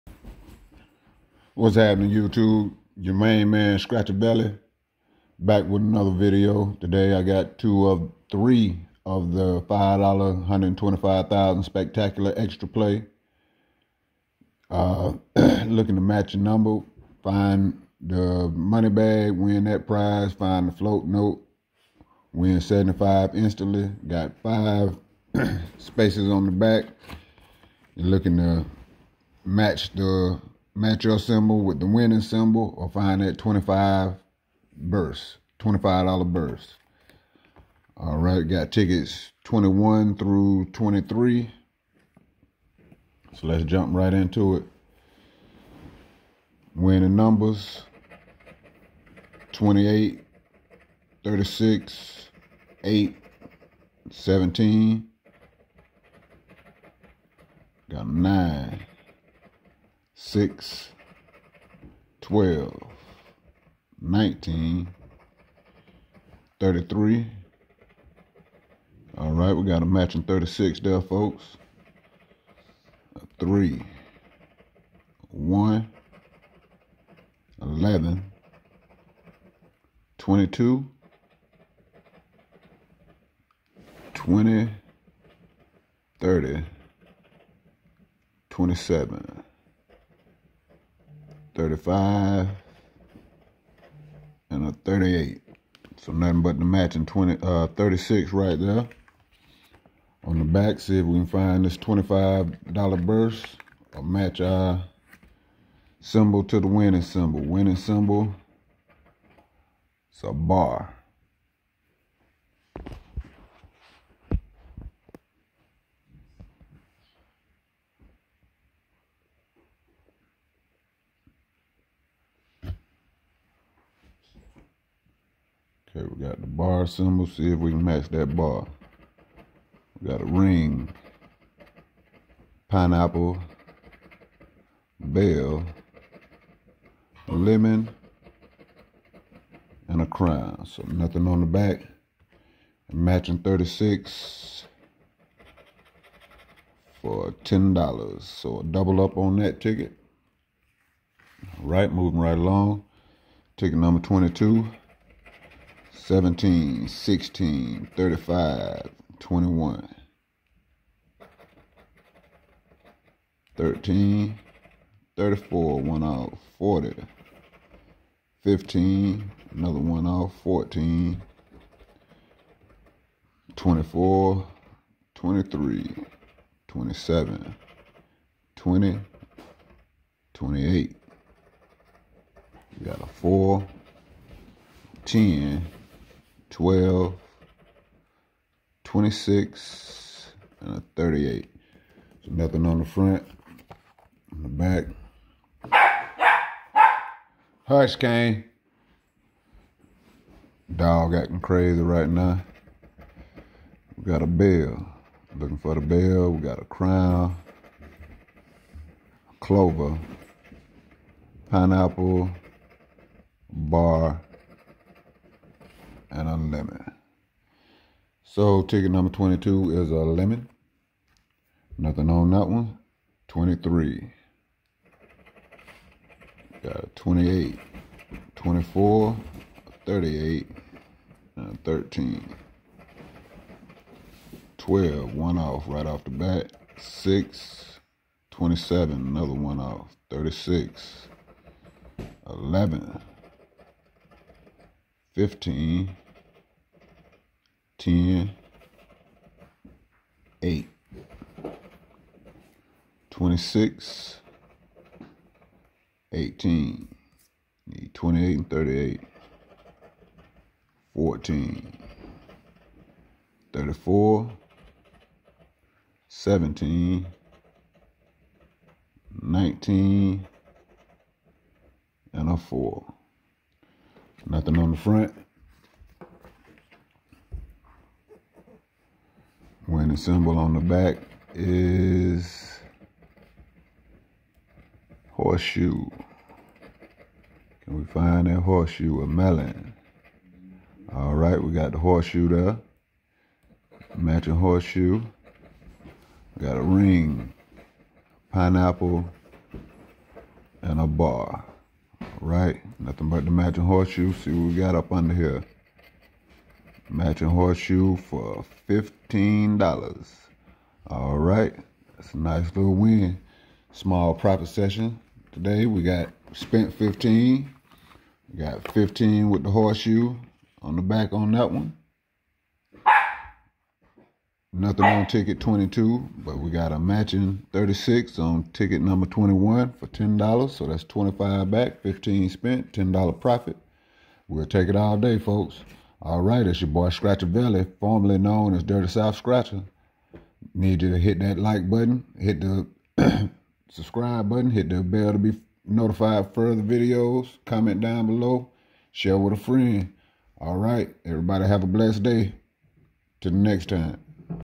A few $5 scratchoff tickets with a little profit. Lottery scratchoff tickets
$5 scratchoff tickets 125,000 spectacular extra play with a back scratch. # Lottery scratchoff tickets